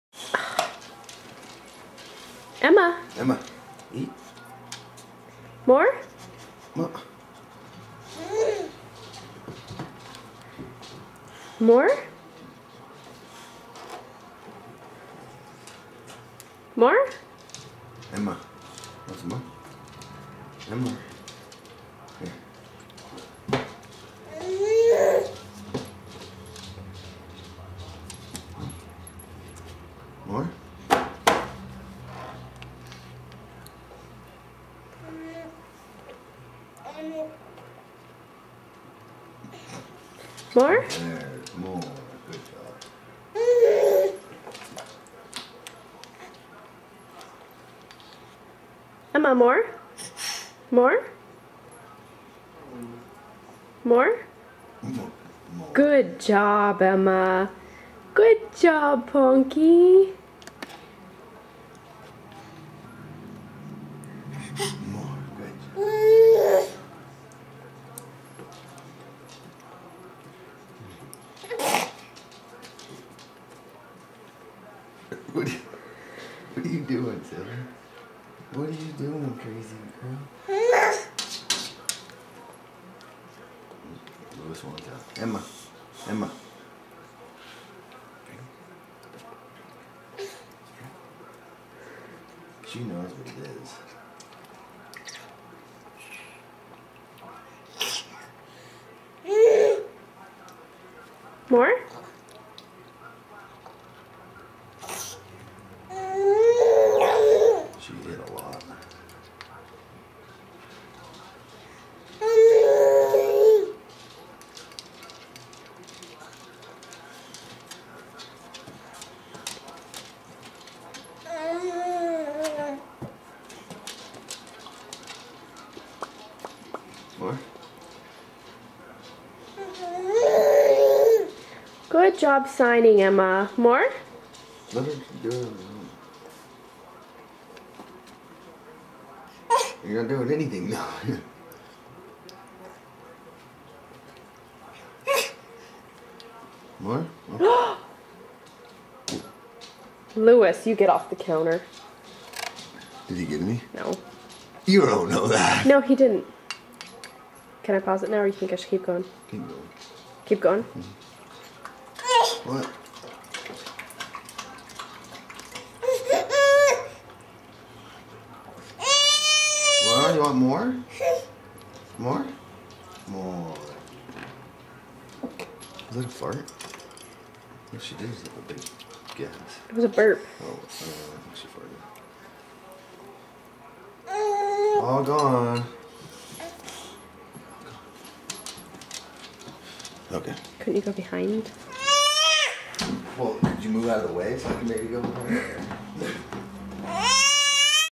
First Time Emma Signs
We had been teaching emma three different signs for about two months.  The more sign was the first sign she learned.